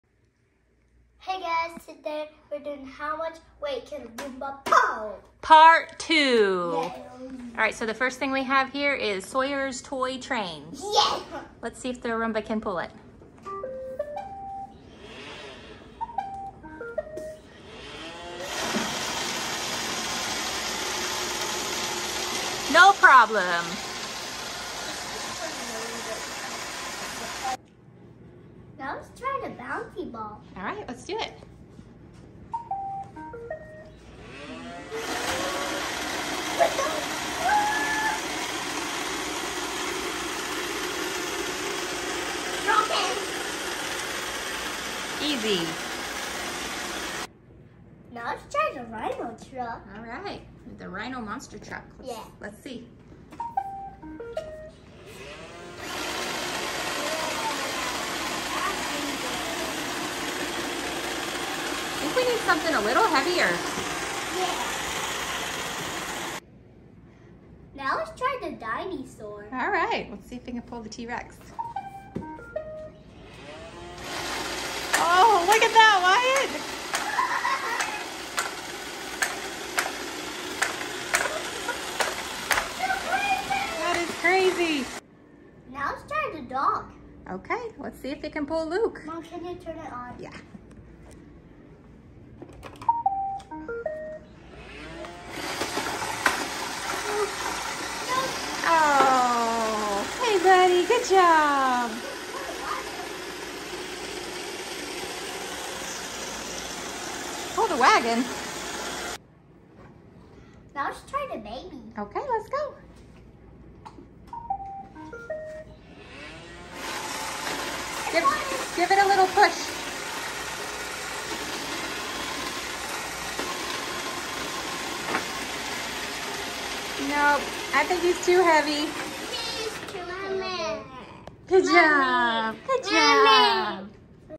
How much weight can a Roomba pull??? Part 2!
Today, we experiment more with how much weight the iRobot Roomba 690 can pull!!!